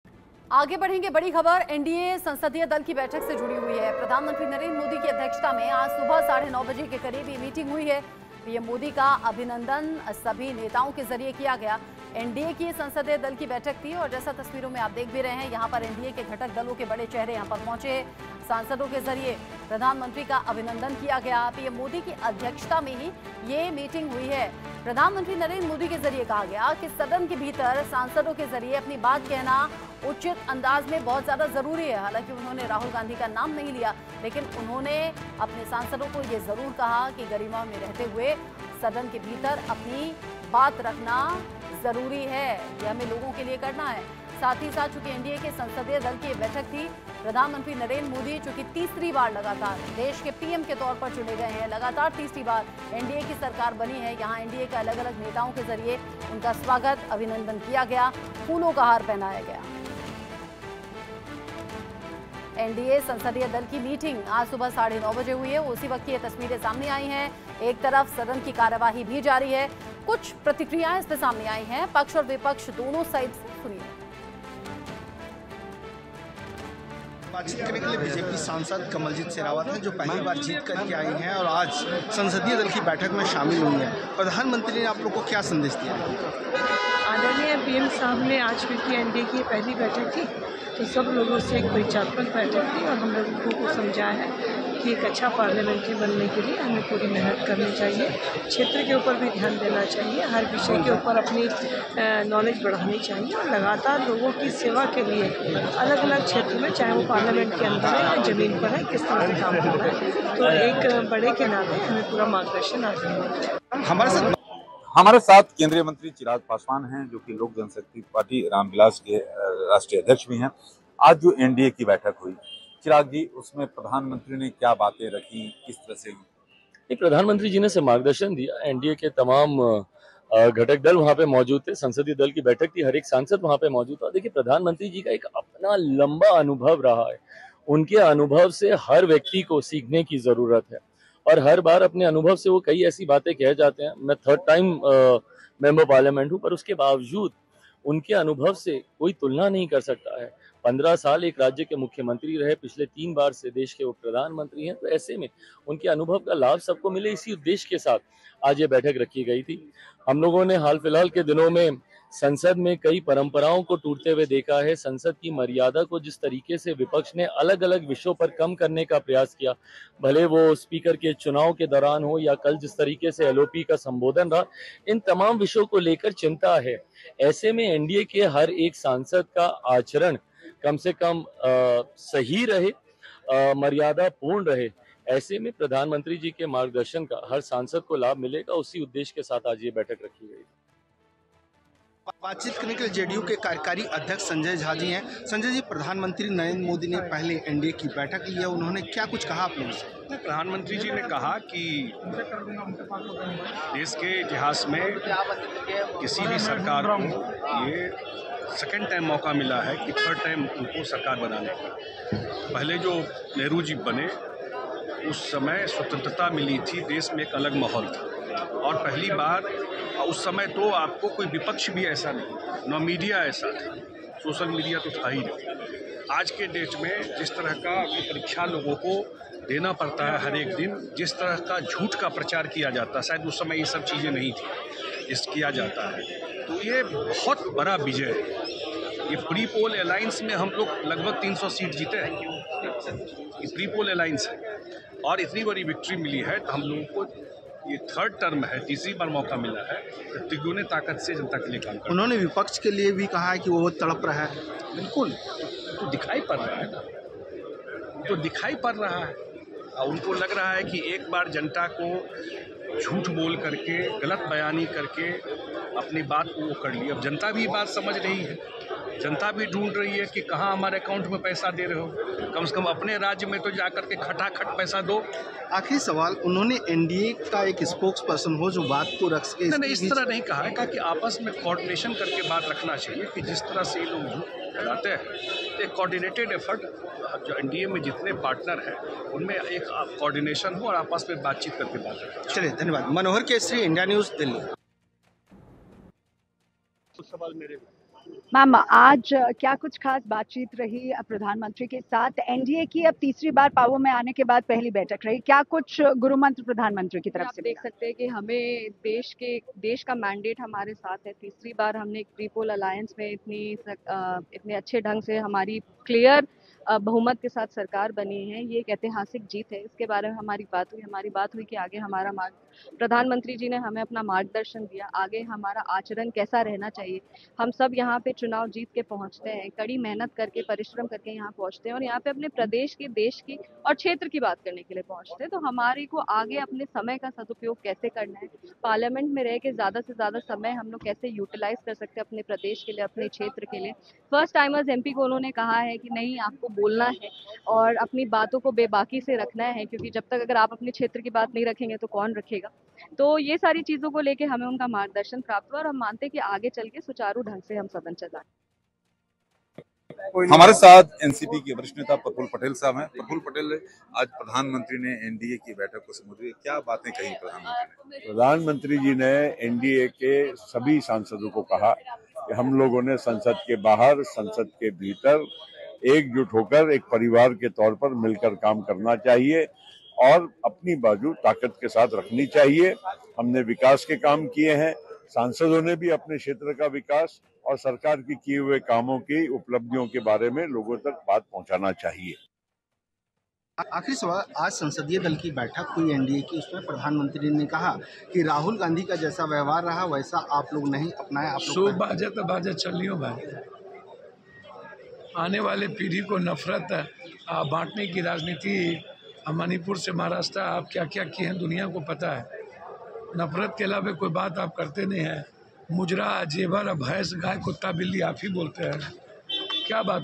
NDA Meeting : प्रधानमंत्री मोदी की अध्यक्षता में NDA की बैठक | PM Modi | India News
आधिकारिक सूत्रों ने बताया कि भाजपा और उसके सहयोगी दलों के सभी सांसदों को मंगलवार की बैठक के बारे में सूचित कर दिया गया है। उन्हें बैठक में शामिल होने के लिए कहा गया है। यह बैठक इसलिए भी अहम है, क्योंकि भाजपा को 2014 के बाद पहली बार हाल ही में हुए चुनावों में लोकसभा में बहुमत नहीं मिला है। सरकार चलाने के लिए पार्टी अपने सहयोगियों पर निर्भर है।

Official sources said all MPs of the BJP and its allies have been informed about Tuesday's meeting. He has been asked to attend the meeting. This meeting is also important because for the first time since 2014, BJP has not got majority in the Lok Sabha in the recent elections. The party is dependent on its allies to run the government.